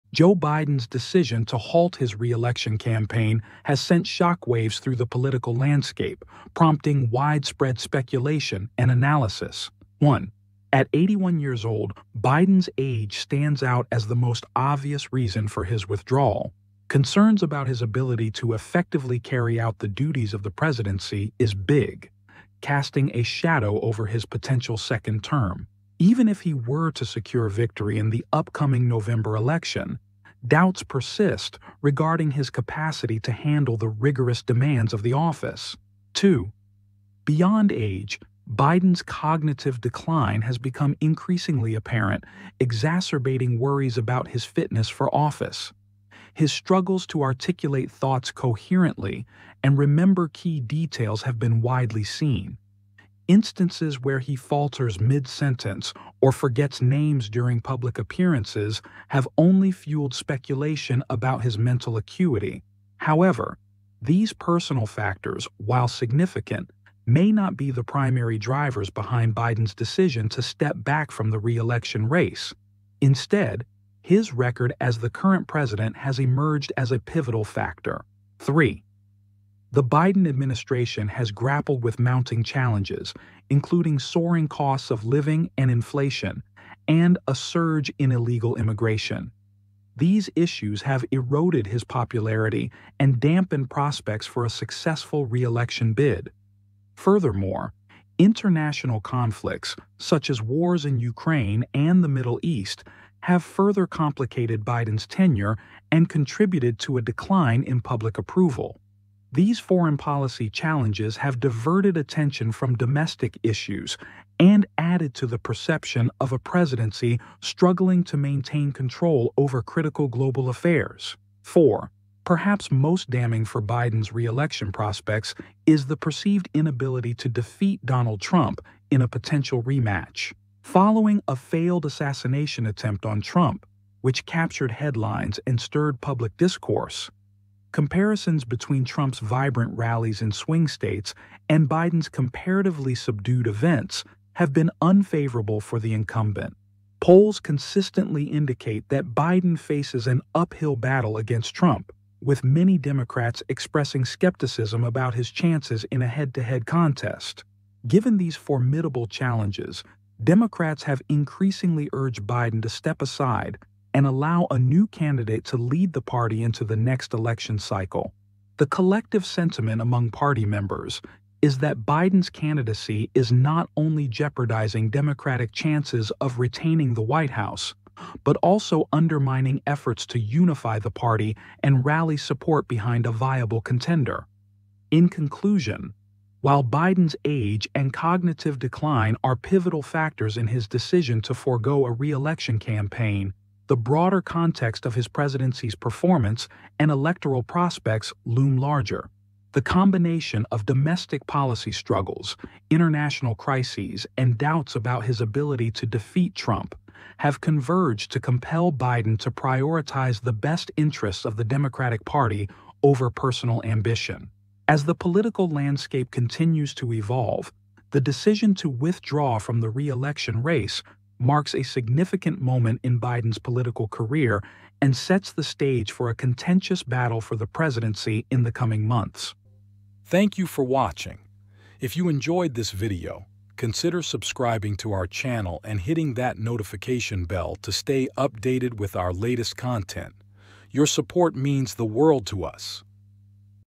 Joe Biden withdraws from US presidential race; these are the main reasons!
Four reasons that why Biden steps down from USA presidential election Candidacy.